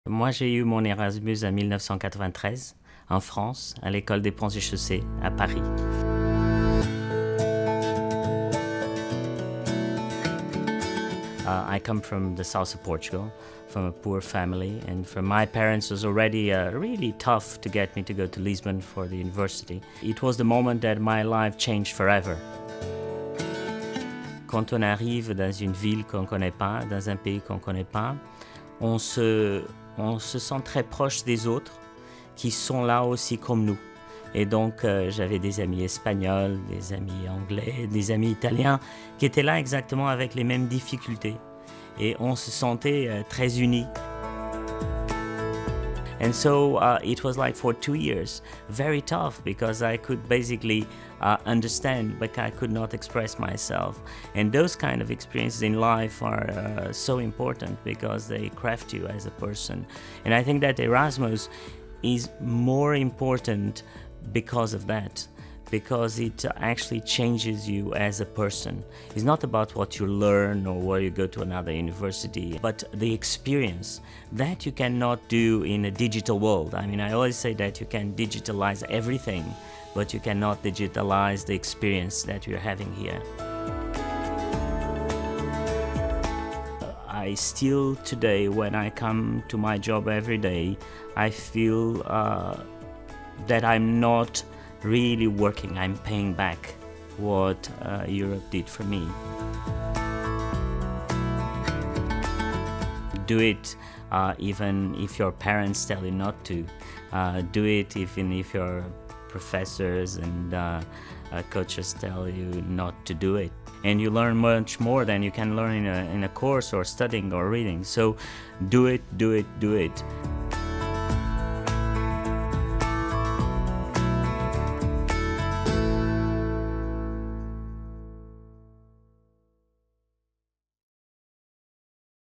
Erasmus generation: Carlos Moedas
This video celebrates the 30th anniversary of the Erasmus programme. Carlos Moedas, European Commissioner for Research, Science and Innovation talks about his Erasmus experience in Paris.